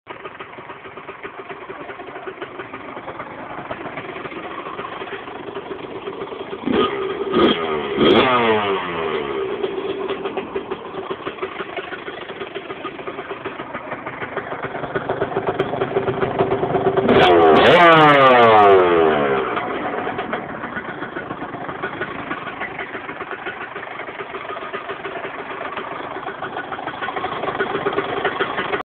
Kx80 Sound ;D
My New bike 1st vid Of it :) round the back of my dads :)